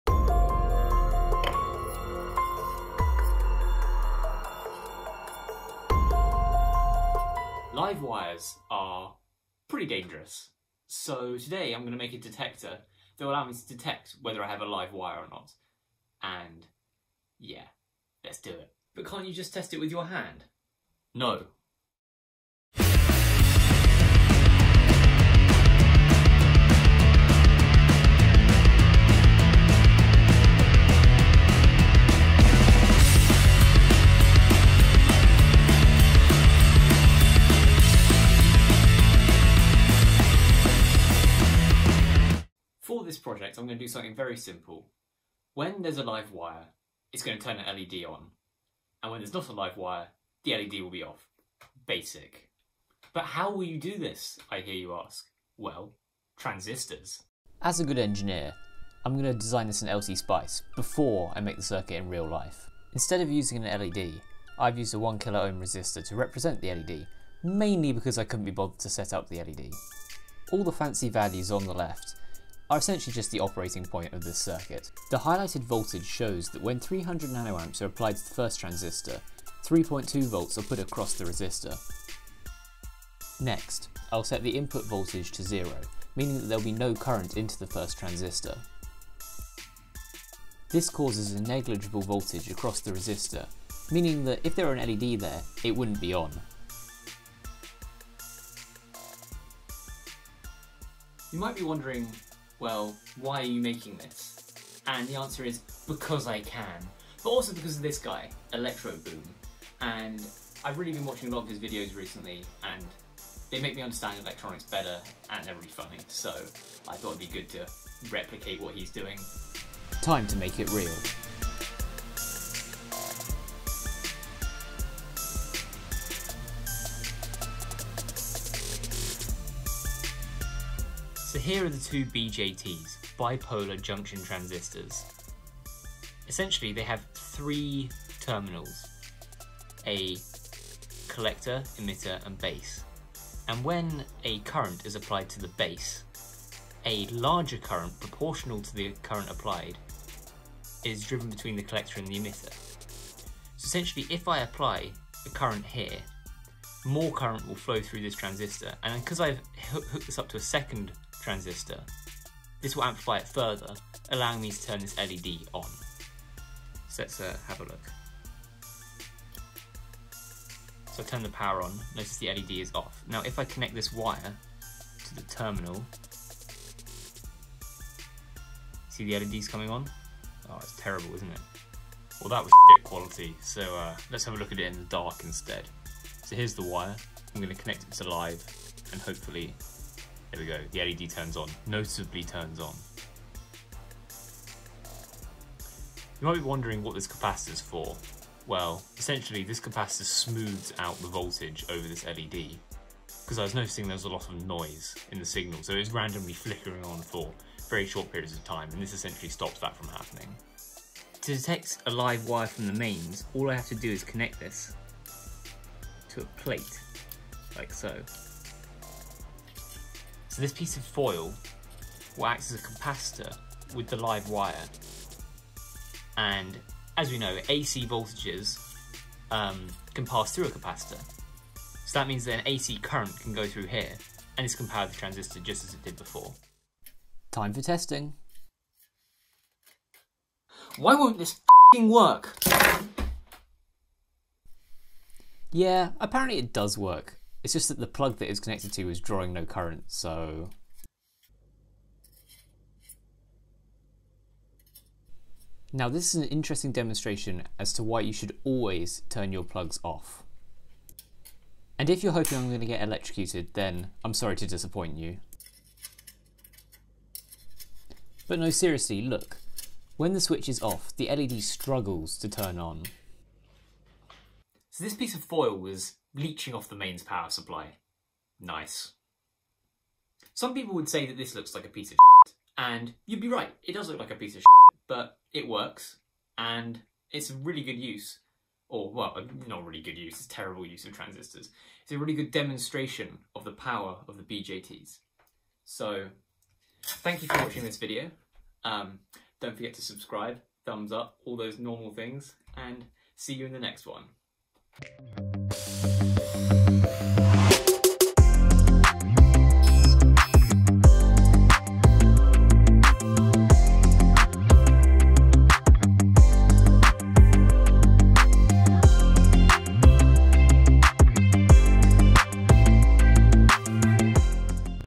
Simple Live Wire Detector
Using a simple transistor circuit to detect live mains wires.

The Dodgy Engineer returns, with regular videos releasing every week from now on! In this video, I design and assemble a circuit using all the transistors I own to turn on an LED when held near a live wire.

This project is fully explained and hopefully easy (and safe!) for others to replicate.

Stay Dodgy!